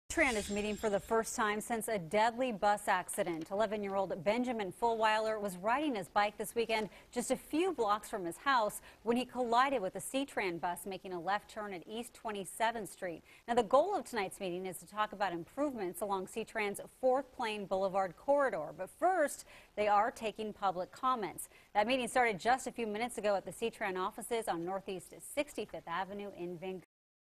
C-Tran eyes change after Vancouver boy's death
Vancouver's transit system officials met for the first time Tuesday since a deadly bus accident. This 11-year-old boy was riding his bike a few blocks from his house when he collided with a C-Tran bus. Find out what you can do to help.